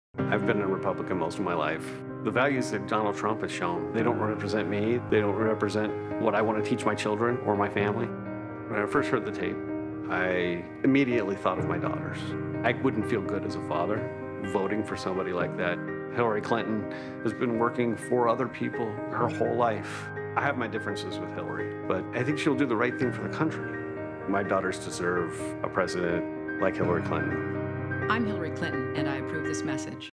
Daughters | Hillary Clinton TV Ad (Terry Daniel)
2016 Presidential Election.
New TV Ad from Hillary Clinton featuring a Republican Voters on Why he's Voting for Hillary.